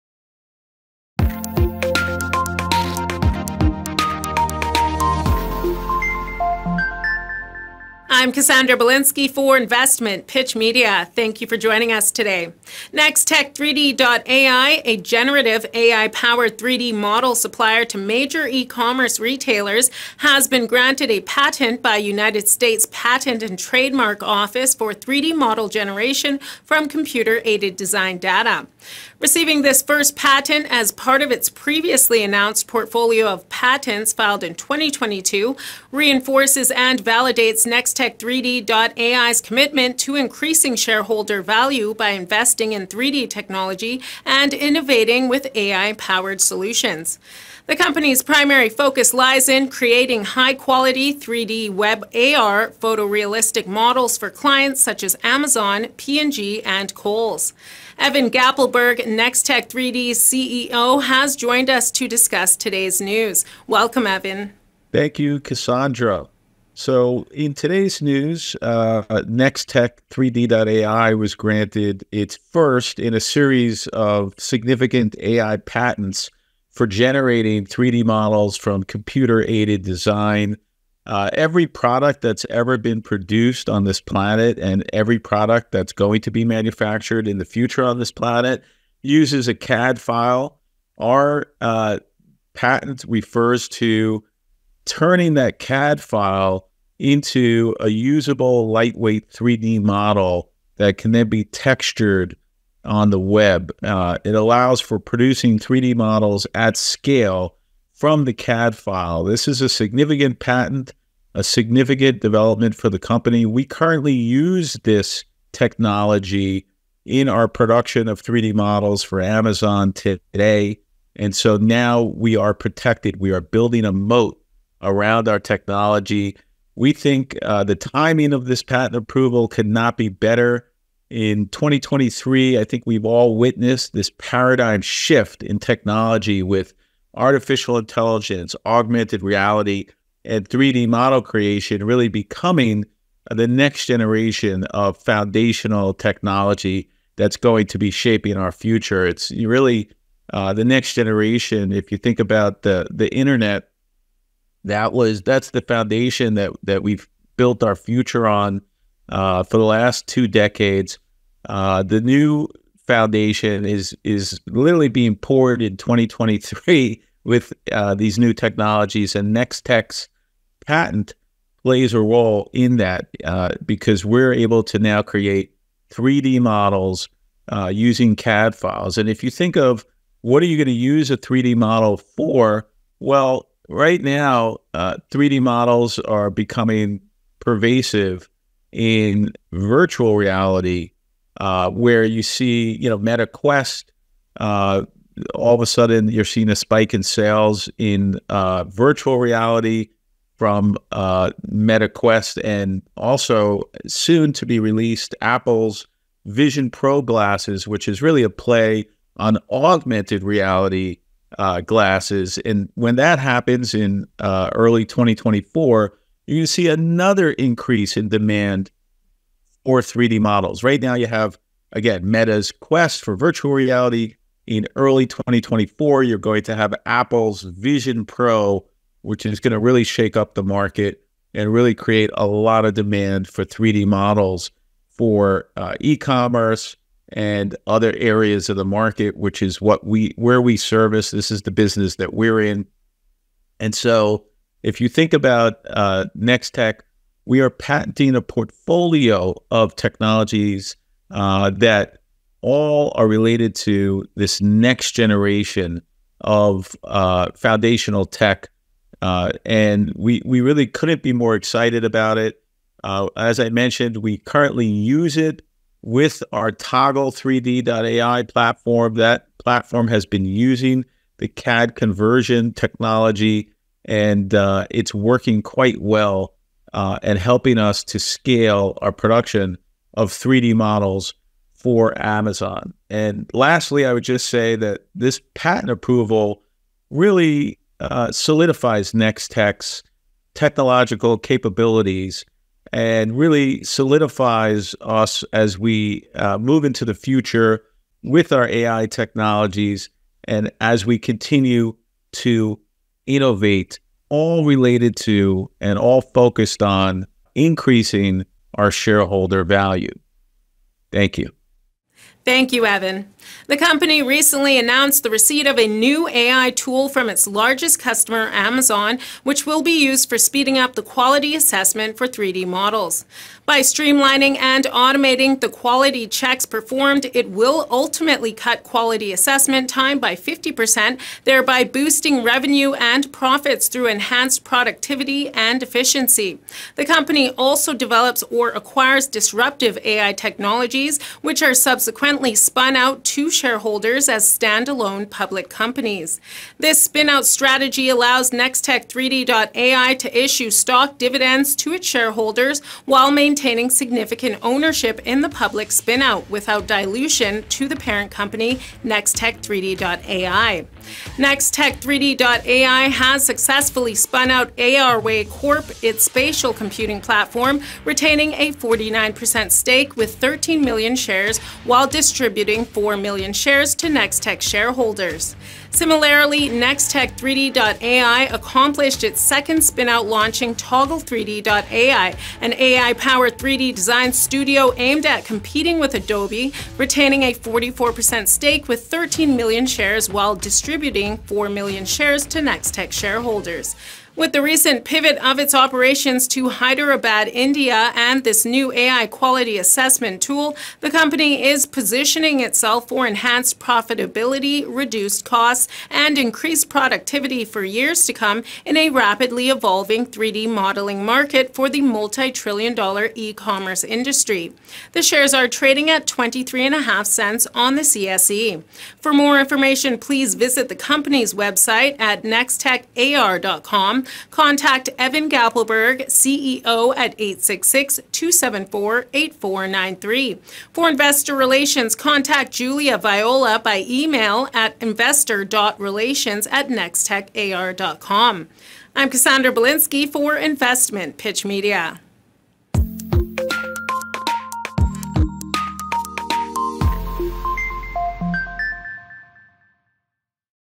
Nextech3D, Generative AI 3D model supplier, has been granted a US patent for 3D CAD model generation
231213
Nextech3D.AI (OTCQX:NEXCF) (CSE:NTAR) (FSE:1SS) (new symbol), a Generative AI-Powered 3D model supplier to major e-commerce retailers, has been granted a patent by United States Patent and Trademark Office for3D model generation from Computer Aided Design (CAD) data.
Receiving this first patent, as part of its previously announced portfolio of patents filed in 2022, reinforces and validates Nextech3D.ai's commitment to increasing shareholder value by investing in 3D technology and innovating with AI-powered solutions.
The company’s primary focus lies in creating high-quality 3D WebAR photorealistic models for clients such as Amazon, P & G, and Kohls.

The company recently announced the receipt of a new AI-tool from its largest customer Amazon which will be used for speeding up the Quality Assessment for 3D models.

By streamlining and automating the quality checks performed, it will ultimately cut quality assessment time by 50%, thereby boosting revenue and profits through enhanced productivity and efficiency.

The company also develops or acquires disruptive AI-technologies, which are subsequently spun out to shareholders as standalone public companies.

This spin-out strategy allows Nextech3D.ai to issue stock dividends to its shareholders while maintaining significant ownership in the public spin-out, without dilution to the parent company Nextech3D.ai.

Nextech3D.ai has successfully spun out ARway Corp (OTCQB:ARWYF) (CSE:ARWY) (FSE:E65) its spatial computing platform, retaining a 49% stake with 13 million shares while distributing 4 million shares to Nextech shareholders.

Similarly, Nextech3D.ai accomplished its second spin-out launching Toggle3D.ai, (OTCQB:TGGLF) (CSE:TGGL) (FSE:Q0C ) an AI-powered 3D design studio aimed at competing with Adobe, retaining a 44% stake with 13 million shares while distributing 4 million shares to Nextech3D shareholders.

With the recent pivot of its operations to Hyderabad India and this new AI Quality Assessment Tool, the company is positioning itself for enhanced profitability, reduced costs, and increased productivity for years to come in a rapidly evolving 3D modeling market for the multi trillion-dollar ecommerce industry.

The shares are trading at $0.235. on the CSE.